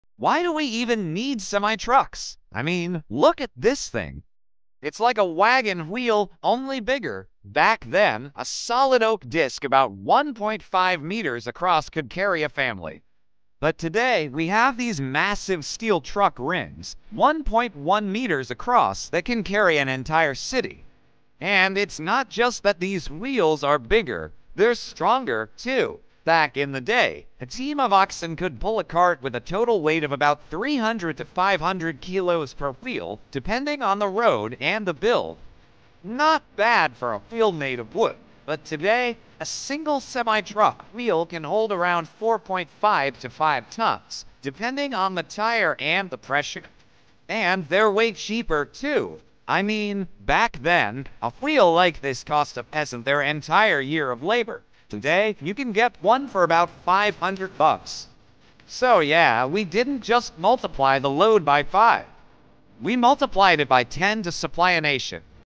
300 KG vs 5 TONS Evolution of the Wheel
300 KG vs 5 TONS — the evolution of the wheel from wooden cart to steel giant. Discover how engineering transformed load capacity from carrying a single family to moving an entire city. History, technology, and the machines that changed transportation forever.

Suggested keywords at the start: wheel evolution, history of transport, truck wheel, cart wheel, engineering, load capacity, StorisOfWheels